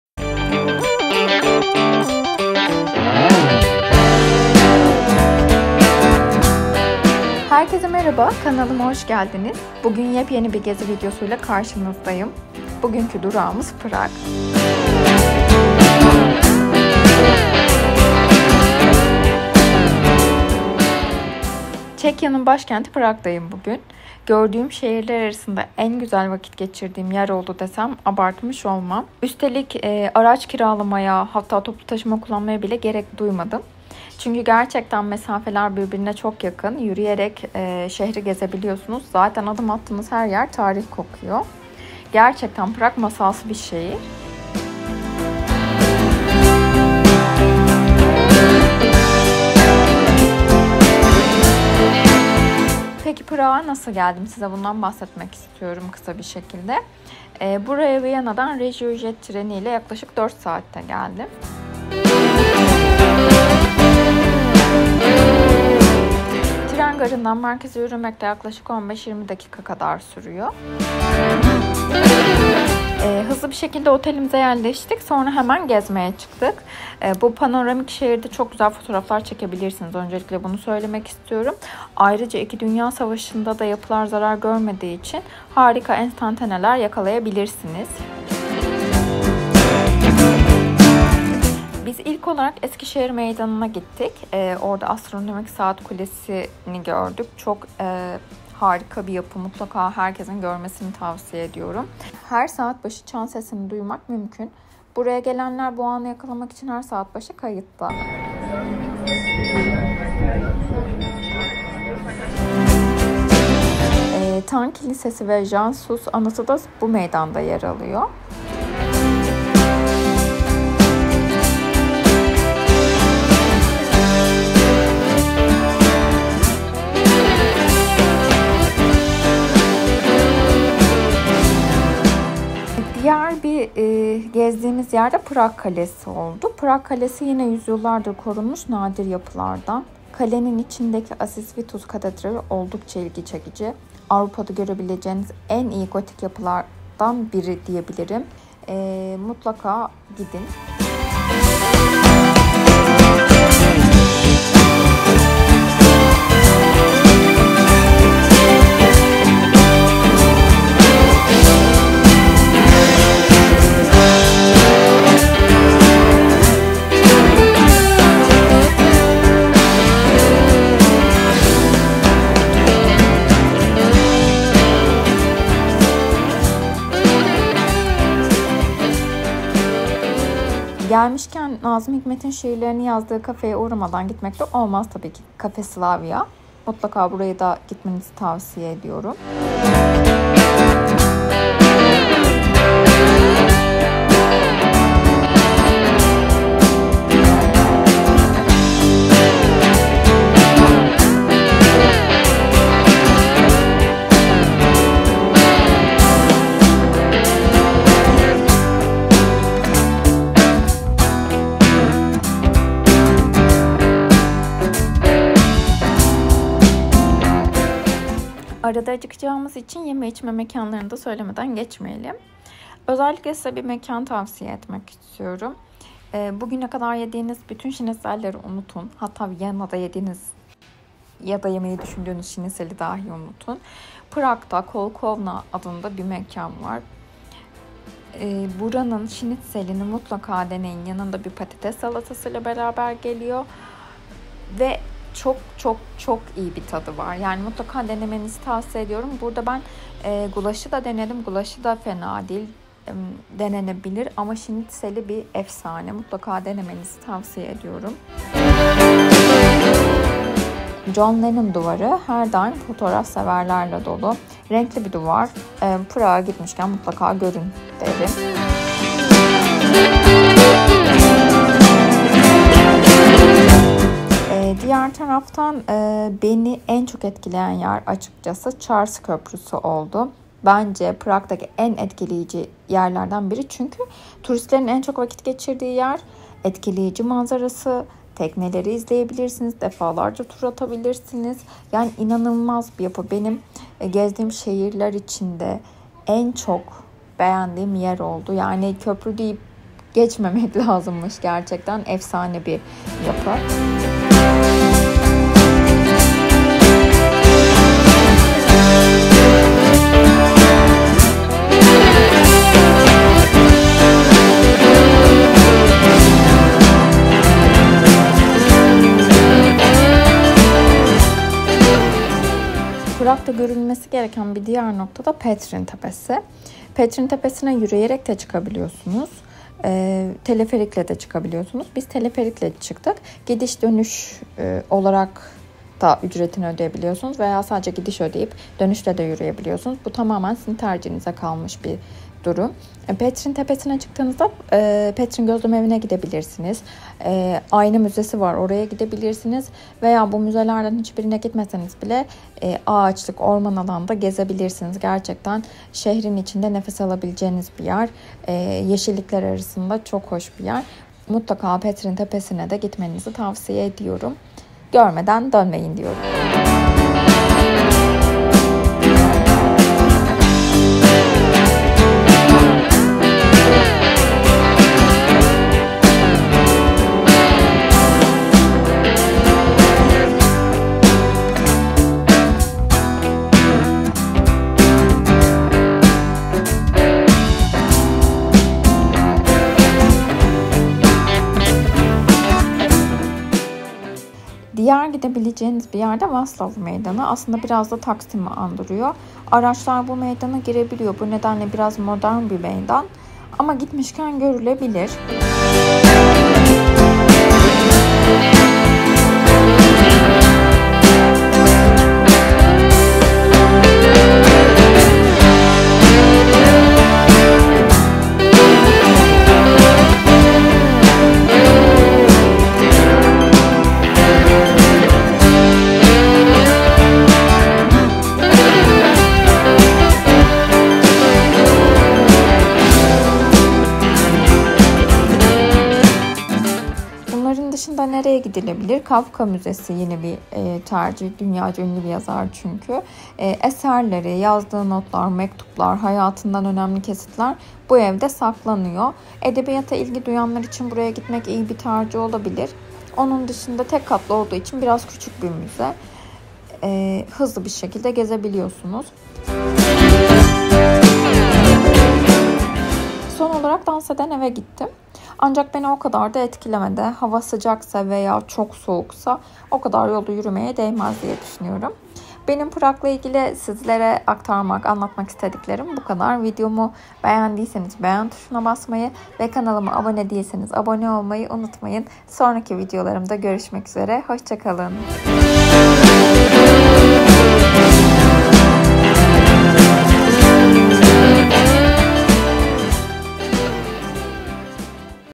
Prag'da ne yapılır? Çekya'yı geziyorum (Nazım Hikmet'in mekânı Slavia Cafe'ye de gittik)
Çekya, eski adıyla Çek Cumhuriyeti, tarihi yapısı ve olağanüstü doğasıyla turistlerin en çok ziyaret ettiği ülkeler arasında başı çekiyor. Biz de Prag'da geçirdiğimiz üç gün boyunca neler yaptığımızı, ne yediğimizi sizlere aktardık.

Astronomik Saat Kulesi'nden, dev katedrallere, Charles Köprüsü'nden Nazım Hikmet'in şiirlerini yazdığı Kafe Slavia'ya, John Lenon duvarından Franz Kafka Müzesi'ne, Petrin Tepesi'nden Prag Kalesi'ne kadar pek çok noktayı gezdik.

Daha önceki içeriklerimizi de izlemeyi unutmayın.